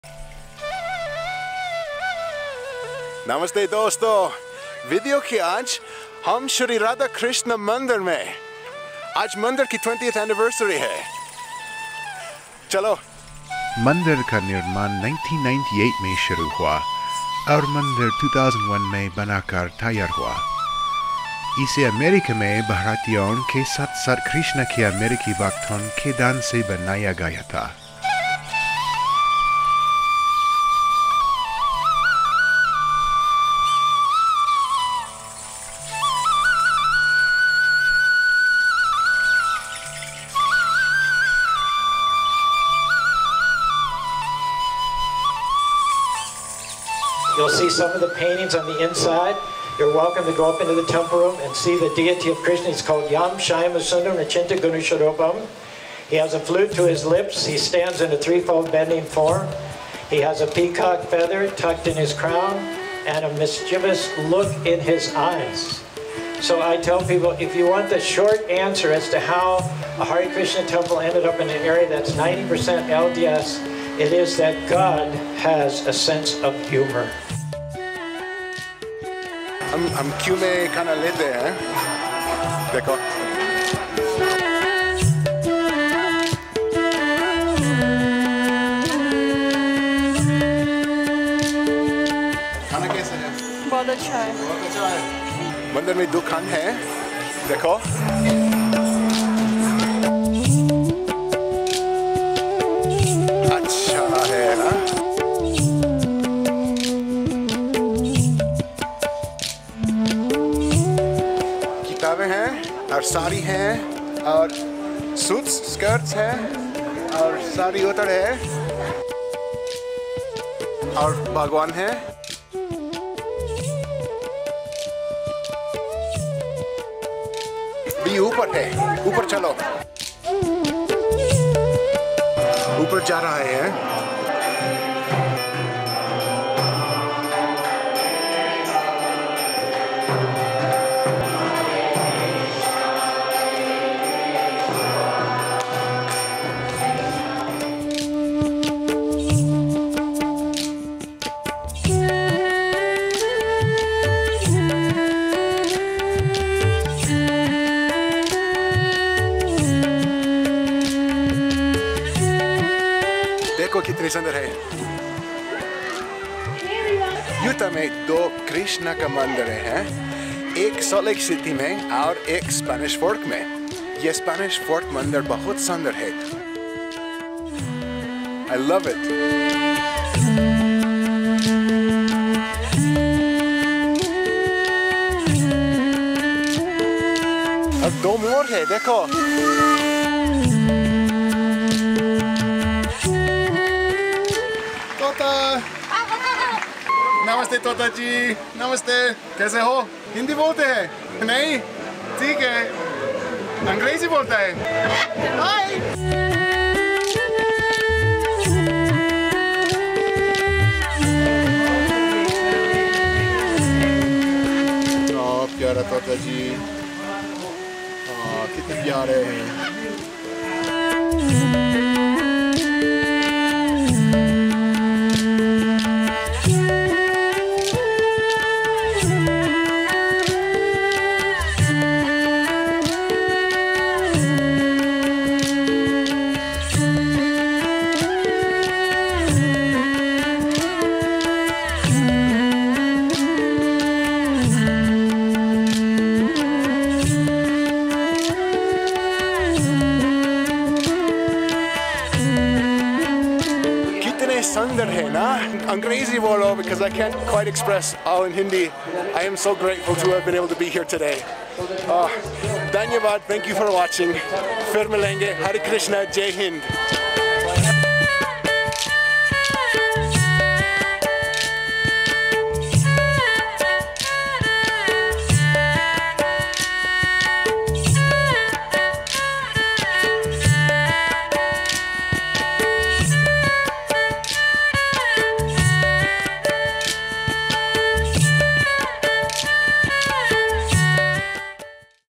Krishna Temple in Mormon Utah!? "God has a sense of humor"
Are you curious about Krishna Temple in Spanish Fork, UT? Watch this video to find out if "God has a sense of humor"

In this video, we check out the Krishna Temple in Spanish Fork, UT – a place of worship for followers of Hinduism, Iskcon (Hindu Religious Society of North America), and other religious affiliations. According to the temple's website, the temple is "dedicated to the worship of the Divine Mother, the embodiment of truth and love."

We'll hear from temple's priest, who tells us about the temple and its community of worshippers. Plus, we see the beautiful Spanish Fork temple in action during its 20th anniversary celebration and learn about its history.

In today's video we visit the Sri Sri Radha Krishna Temple in Spanish Fork, Utah, USA for the 20th anniversary celebration of its construction!
Construction of the temple began in 1998 and the temple was completed in 2001.
It was built by donations from Indians in America as well as American devotees of Krishna.

This is just one of a small handfull of Hindu Temples in Utah!

Sri Sri Radha Krishna Temple 20th Anniversary, Spanish Fork Utah - Foreigner Speaking Hindi | ISKCON

Aaj ke video mein ham Spanish Fork, Utah, USA mein shree shree raadha krshn mandir ke nirmaan kee 20veen varshagaanth samaaroh ke lie jaate hain!
Mandir ka nirmaan 1998 mein shuroo hua tha aur mandir 2001 mein banakar taiyaar hua.
Ise amerika mein bhaarateeyon ke saath-saath krishna ke amerikee bhakton ke daan se banaaya gaya tha.
Maaph keejiye, meri Hindi bahut zyadaa achee nahi 😞

Hare Krishna and Jai Hind!

noSmallCreator - Believe in yourself!